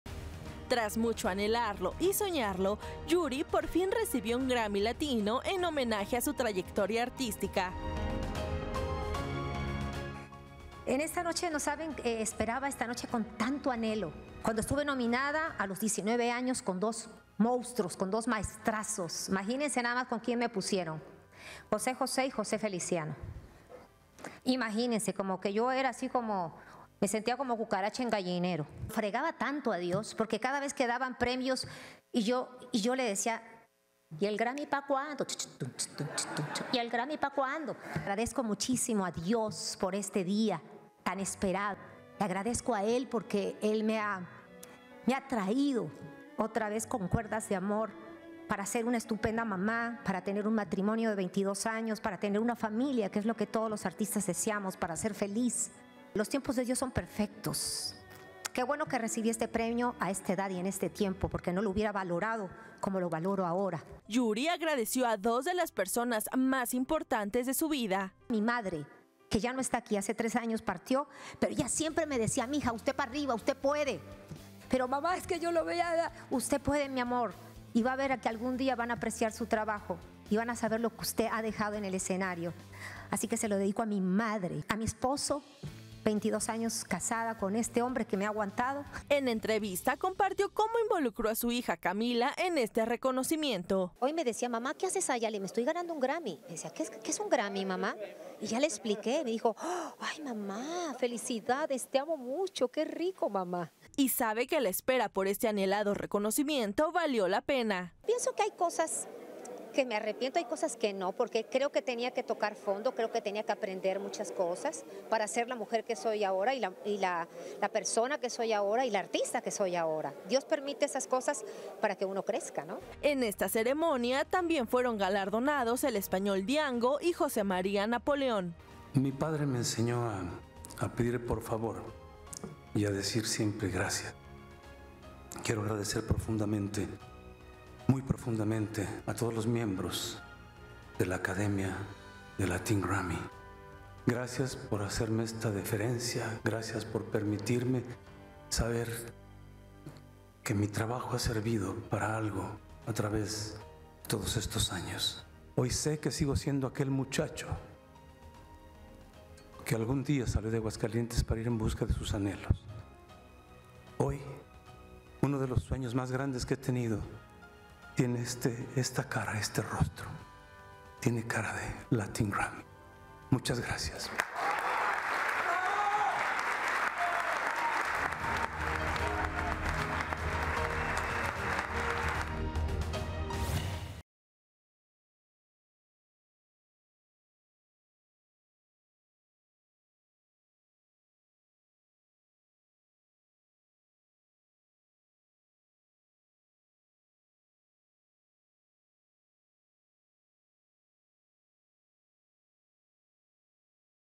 ¡SE LE HIZO JUSTICIA Y ESTÁ FELIZ! Yuri por fin recibió un premio Latin Grammy
La cantante siempre había soñado con recibir un Grammy y agradece haberlo recibido a esta edad porque antes no lo hubiera valorado.